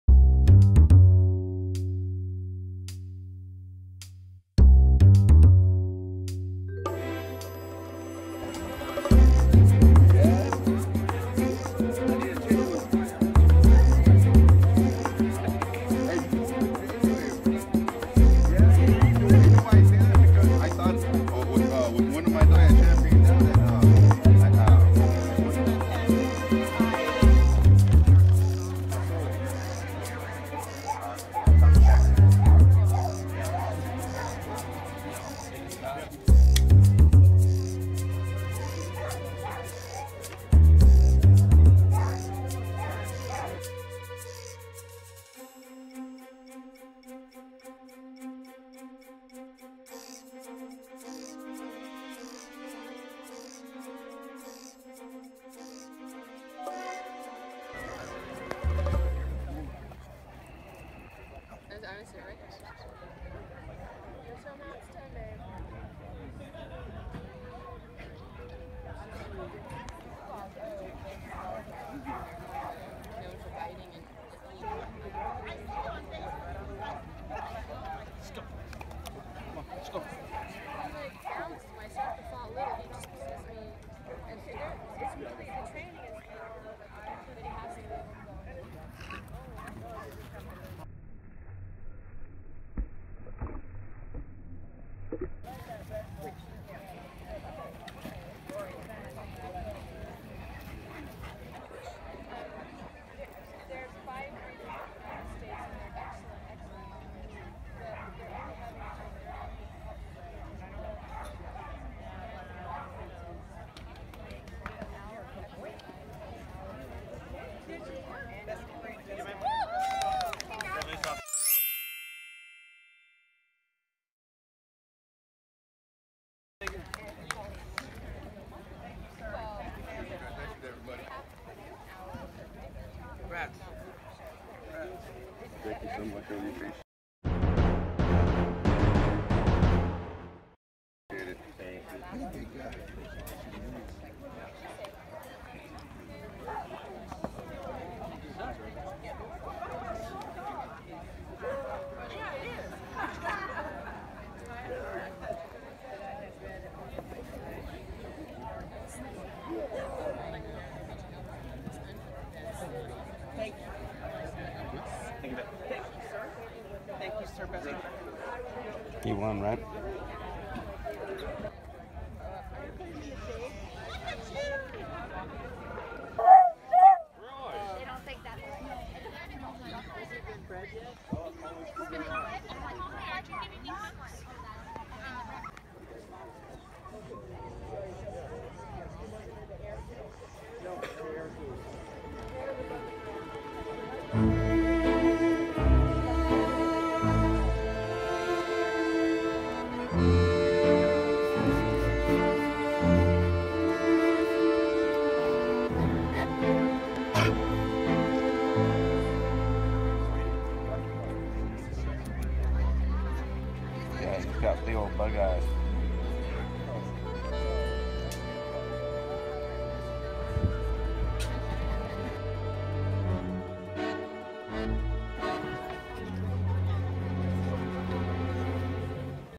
Cane Corso Dog Show: The Champion is Ranked #2 in the USA
Can you guess which one won?? These are the Cane Corso's which were in a Santa Barbara Kennel Club Dog Show in 2019. The champion of this show is, supposedly, ranked the second best Cane Corso in the USA (according to the  contestants that I talked to).

The answer is reveled 2 minutes in.

*This channel exists in order to educate people about dog behavior.*